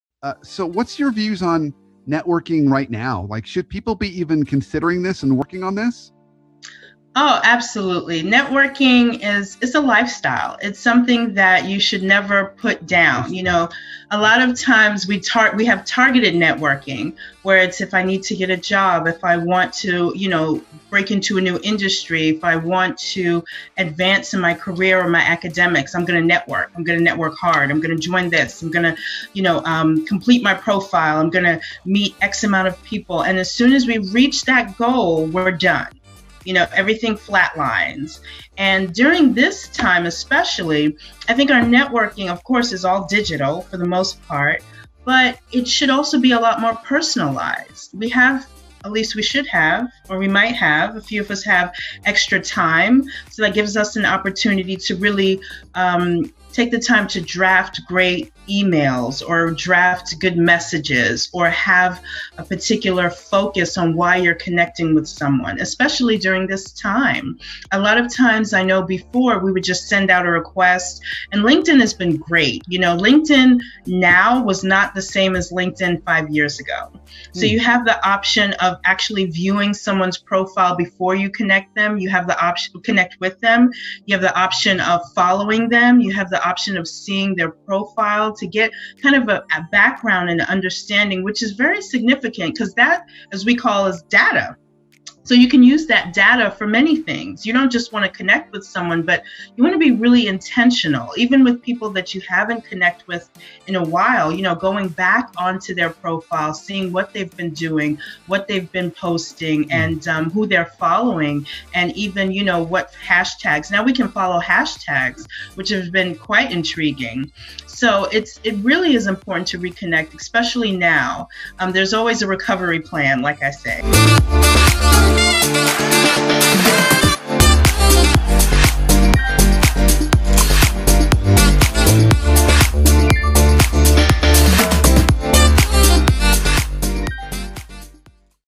Networking During COVID 19 with Chi Chi Okezie
Brandon and Chi Chi Okezie talk about if we should continue networking during quarantine and if so, how we handle it.